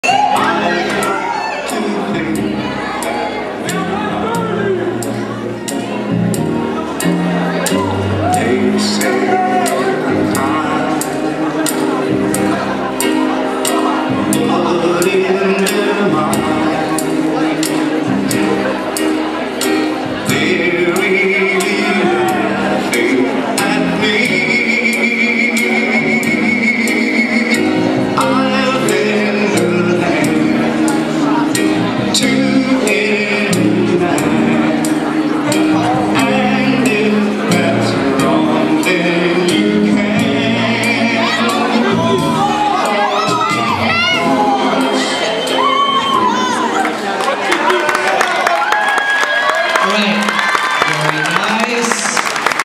Holy Cross 2010 Bronze American Waltz - Round 2, Heat 1
The American Smooth section of the 2010 Holy Cross Ballroom Dance Competition.